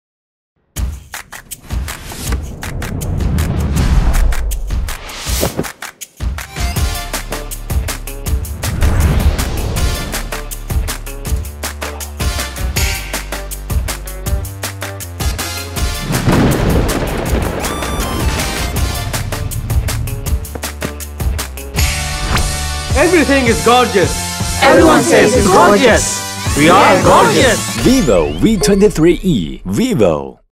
Everyone says it is Gorgeous| V23e | vivo Bangladesh
Everything is Gorgeous ! Everyone says it is Gorgeous!!
The new vivoV23e is coming soon with
50MP AF Portrait Selfie Camera | 7.36mm Ultra Slim AG Design
64MP Night Camera | 44W FlashCharge
Keep your eyes open on 7th February 2022.